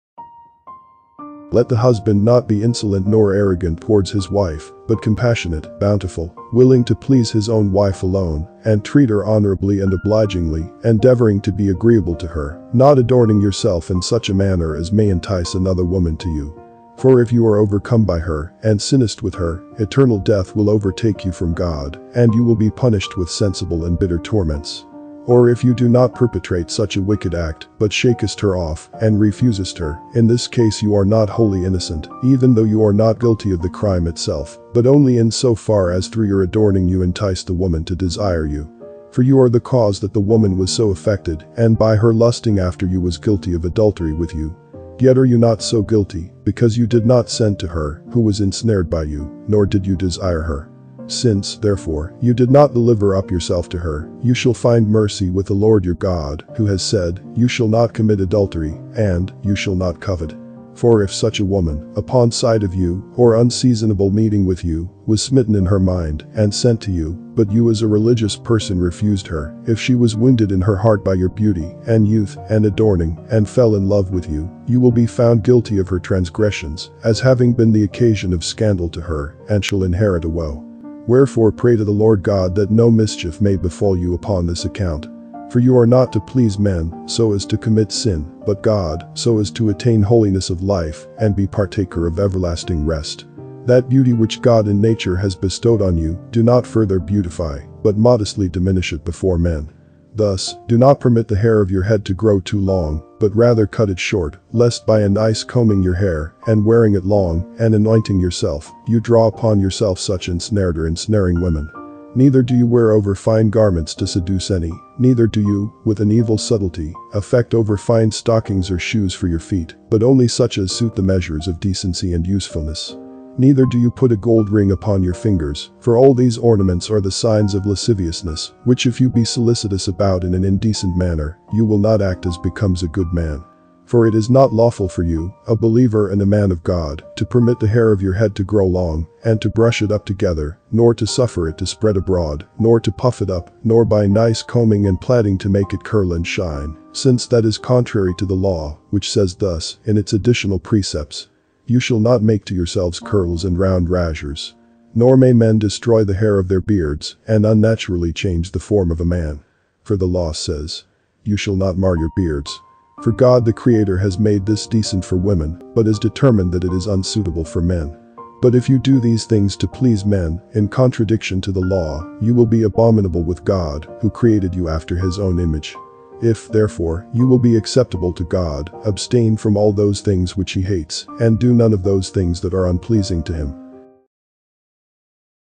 How Should A Christian Dress? - Apostolic Constitutions - Instructions From The Ancient Christians
The apostles chosen by Christ left behind teachings which they intended their followers to adopt. These did not make their way into the Bible. They are instead recorded in a series of books called the Apostolic Constitutions. This is an excerpt from the first book.

———SOCIAL MEDIA———